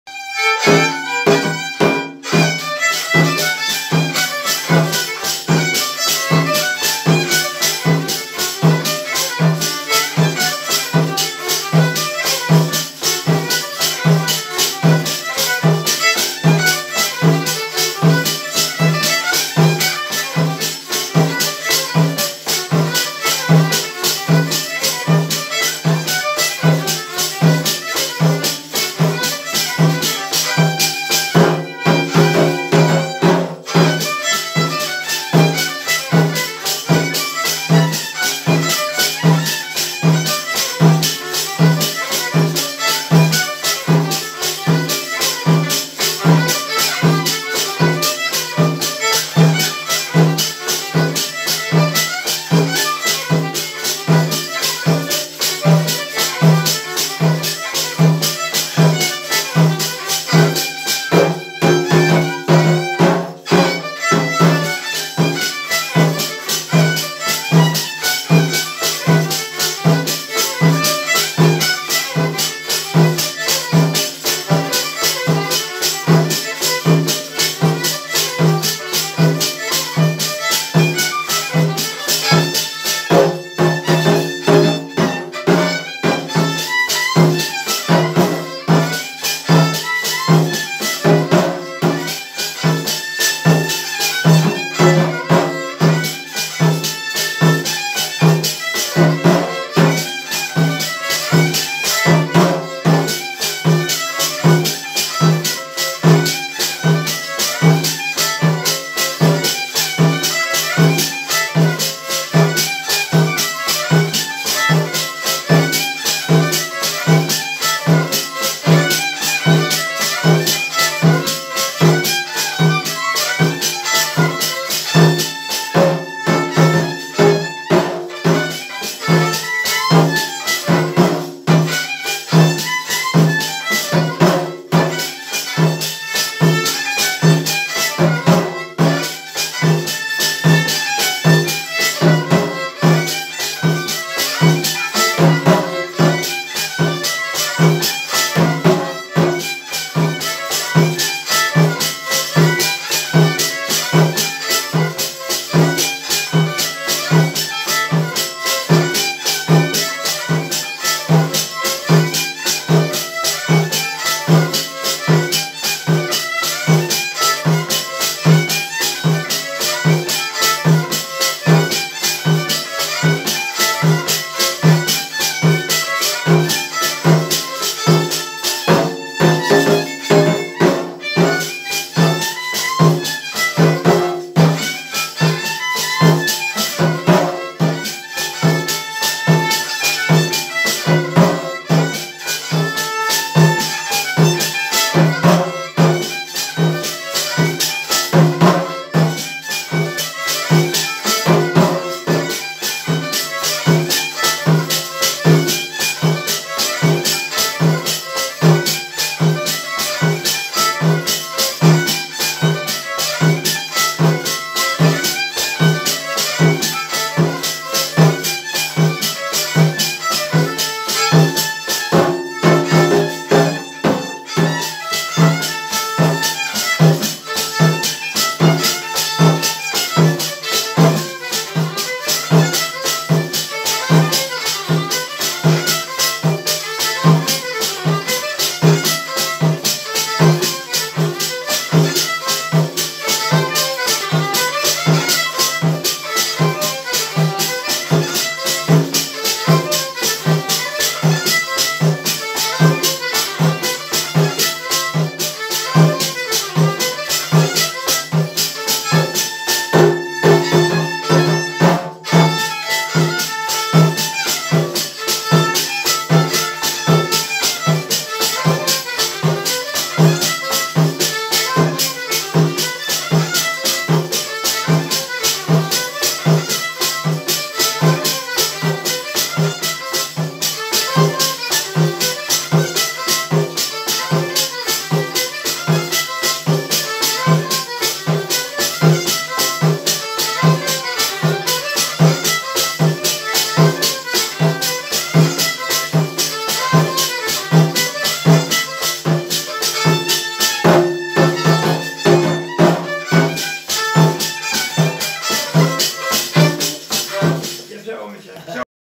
2019 10 13 Przystałowice Małe 8
Grają: Jan Kmita (z Przystałowic Małych) - skrzypce oraz Stanisław Bielawski (z Wiru) - bębenek obręczowy. Kapela Jana Kmity z Przystałowic Małych.